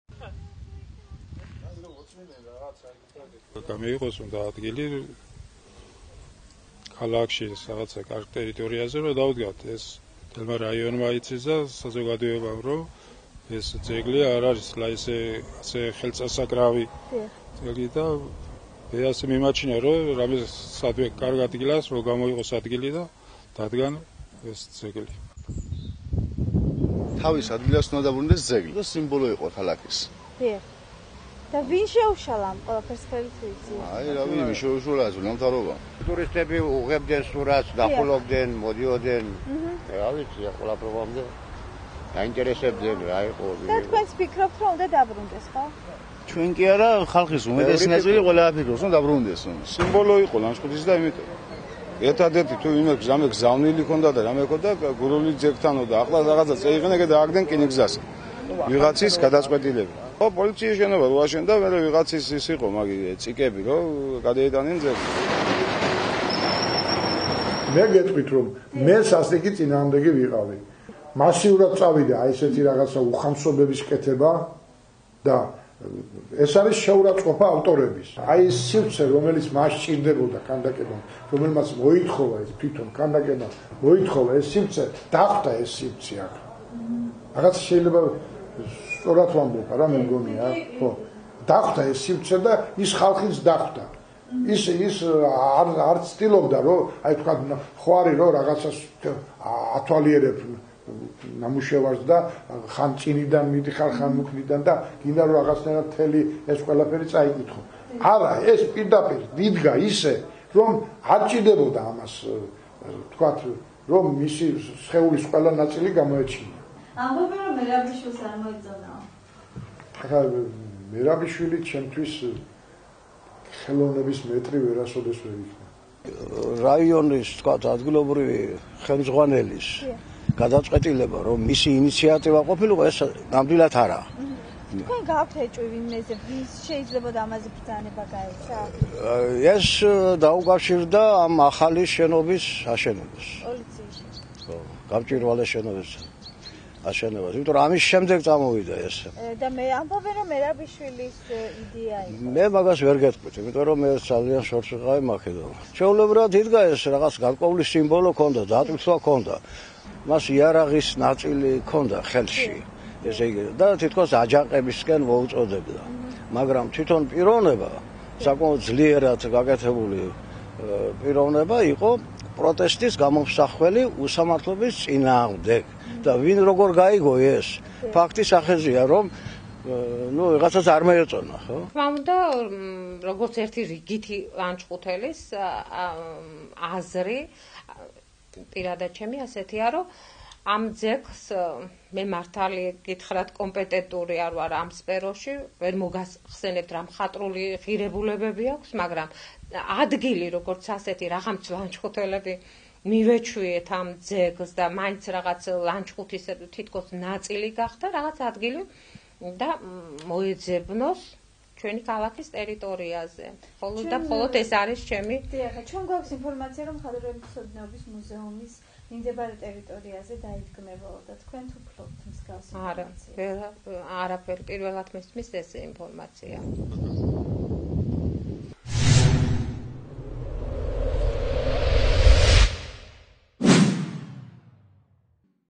„აჯანყებული გურულის" ძეგლი
ლანჩხუთში პოლიციის გამჭვირვალე შენობას „აჯანყებული გურულის" ძეგლი შეეწირა. ლანჩხუთის ცენტრში პოლიციის შენობის გახსნიდან რამდენიმე დღეში გოგი ოჩიაურის „აჯანყებული გურული" ღამით ჩუმად მოხსნეს და რკინიგზის ტერიტორიაზე დააგდეს.